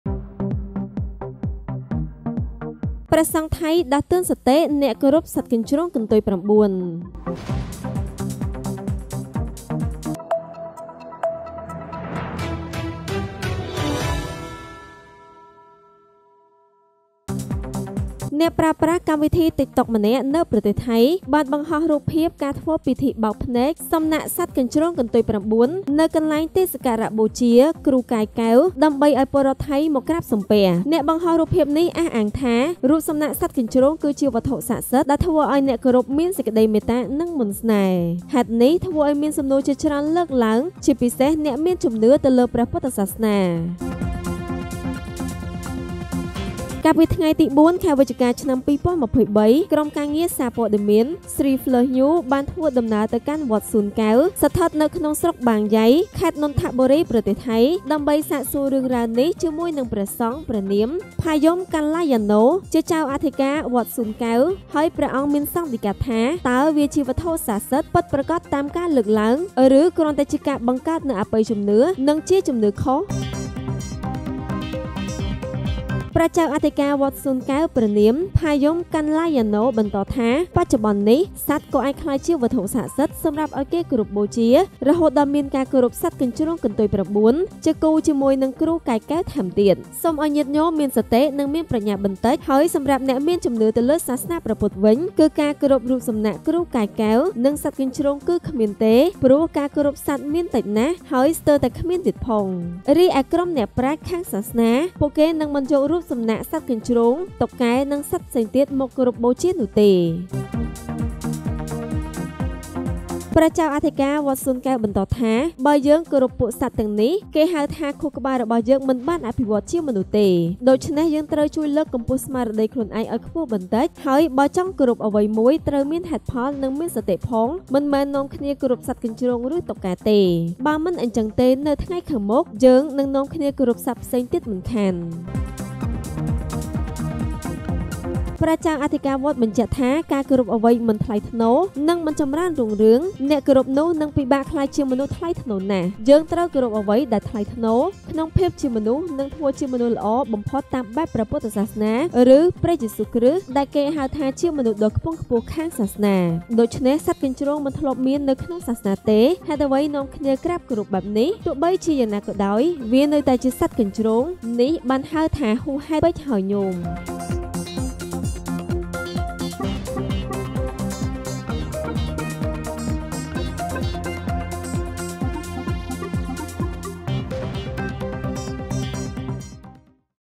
ព្រះសង្ឃថៃដាស់តឿនសតិអ្នកគោរពសត្វកញ្ជ្រោងកន្ទុយ៩
ប្រទេសថៃ ៖ អ្នកប្រើប្រាស់កម្មវិធី TikTok ម្នាក់នៅប្រទេសថៃបានបង្ហោះរូបភាពការធ្វើពិធីបើកភ្នែកសំណាកសត្វកញ្ជ្រោងកន្ទុយ៩ នៅកន្លែងសក្ការៈបូជាគ្រូកាយកែវ ដើម្បីឱ្យពលរដ្ឋថៃមកក្រាបសំពះ ។ អ្នកបង្ហោះរូបភាពនេះអះអាងថា រូបសំណាកសត្វកញ្ជ្រោង គឺជាវត្ថុស័ក្តិសិទ្ធិដែលធ្វើឱ្យអ្នកគោរពមានសេចក្តីមេត្តា និងមន្តស្នេហ៍ ។ ហេតុនេះ ធ្វើឱ្យមានសំណួរជាច្រើនលើកឡើង ជាពិសេស អ្នកមានជំនឿទៅលើសាសនាព្រះពុទ្ធ ។